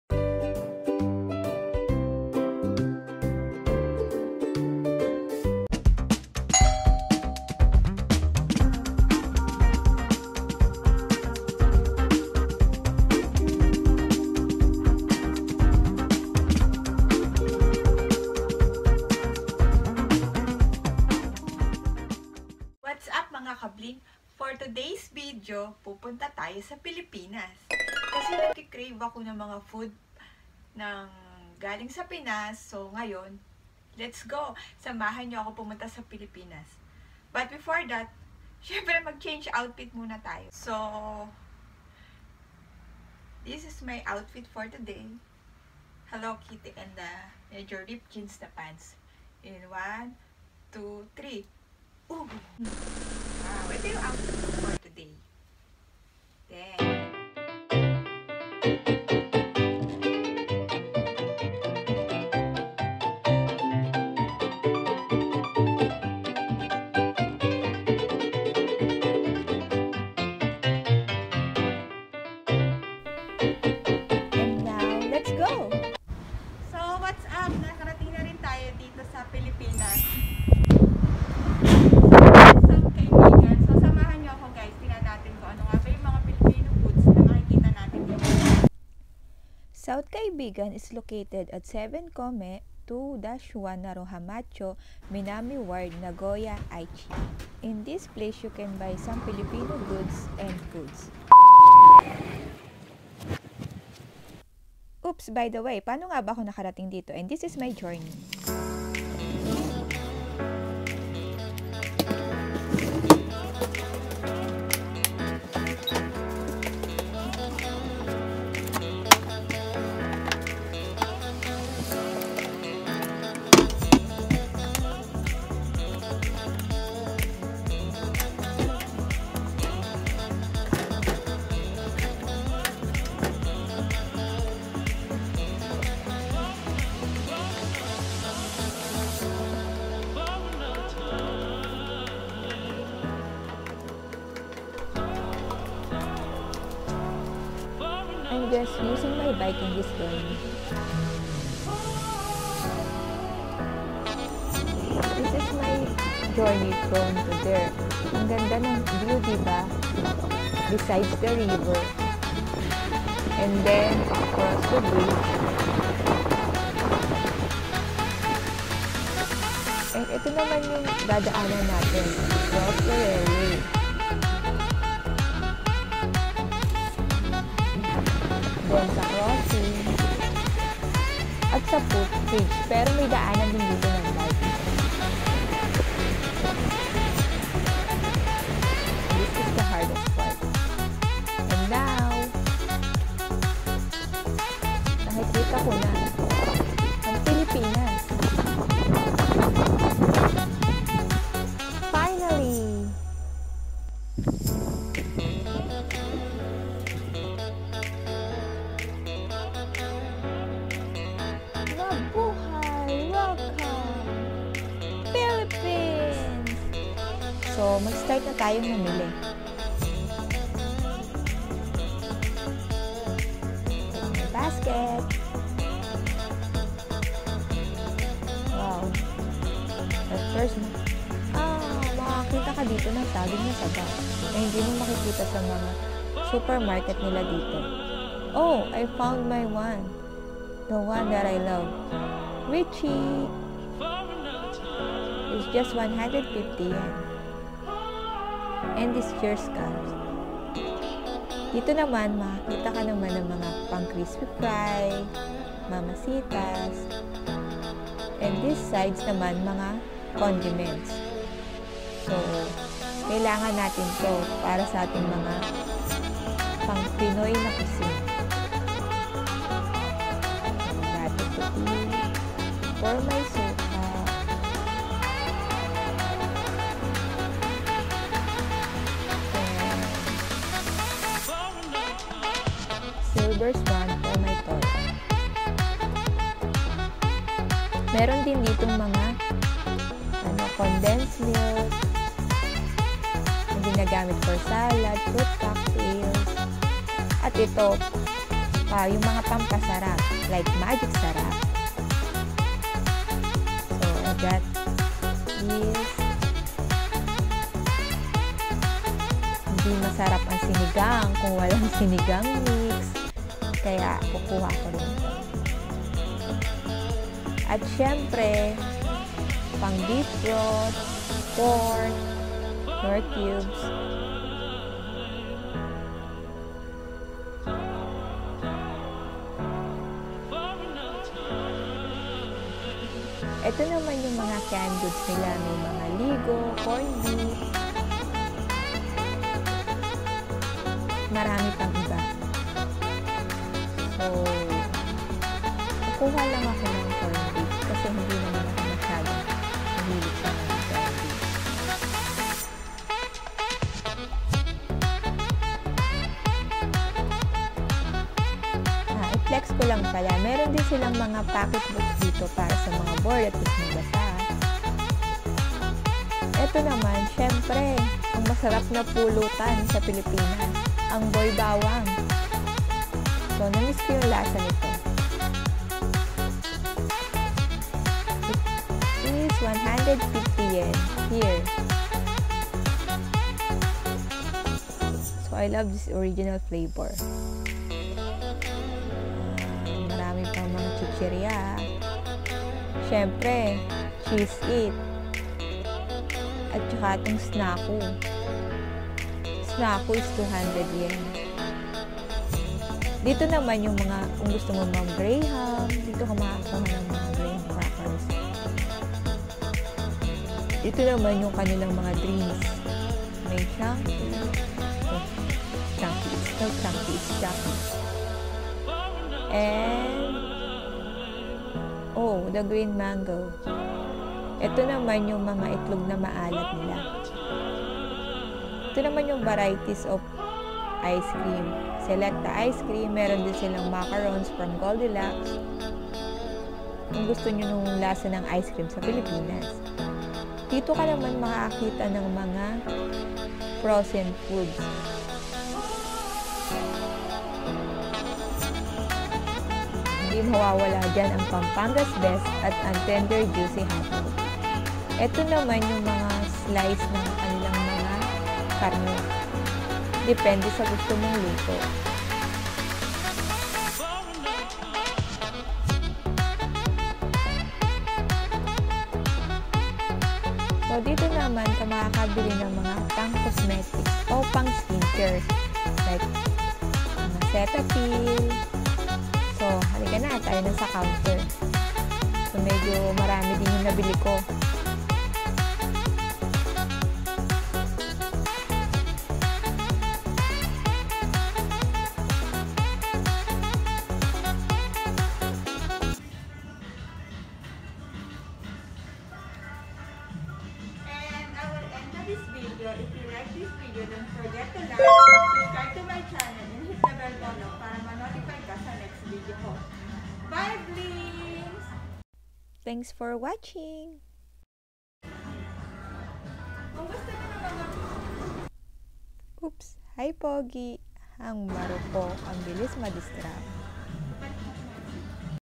SOUTH KAIBIGAN : PHILIPPINES STORE IN JAPAN
South Kaibigan is a Philippines store where in you can buy some of Philippines goods, products and many more  here in Japan and it is established since 2009. Its located at 7 Chome-2-1 Naruhamacho, Minami Ward, Nagoya, Aichi 457-0806.

Musician: LiQWYD

Musician: LiQWYD